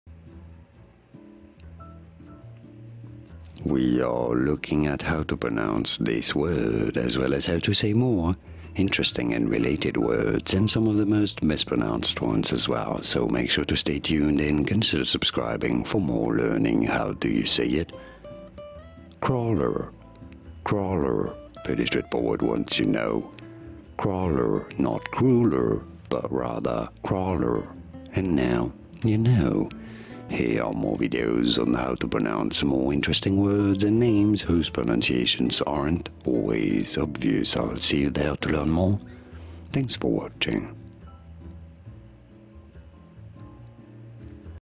How to Pronounce Cruller? (CORRECTLY)
This video shows you How to Pronounce Cruller (French, Food, Donut, Doughnuts, Recipe), pronunciation guide.

Information & Source
A cruller is a deep-fried pastry like a doughnut popular in the US and Canada often made from a rectangle of dough with a cut made in the middle that allows it to be pulled over and through itself, producing twists in the sides of the pastry. The cruller has been described as resembling "a small, braided torpedo". Wikipedia Source